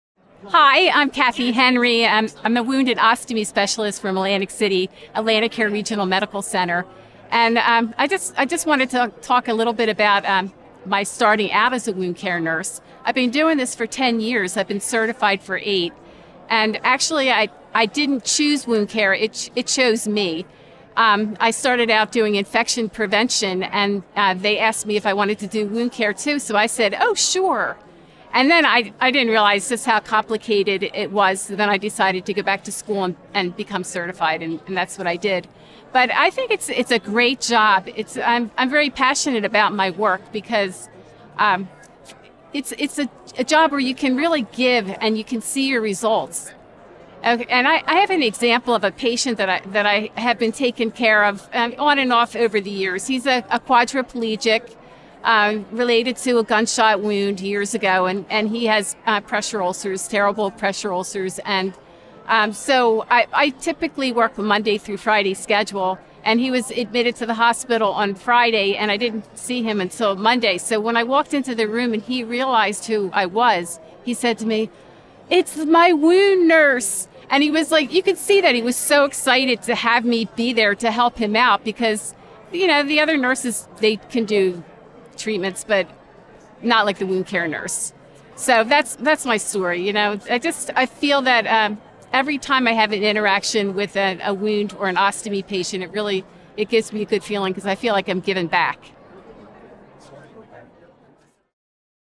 A Passion For Results | Cathleen Henry | Nurse Story
Cathleen Henry is one of the many WOC Nurses that feels like she was chosen by the field instead of the other way around. She loves being able to give back to her patients in a specialty where you can see results and patients feel the difference.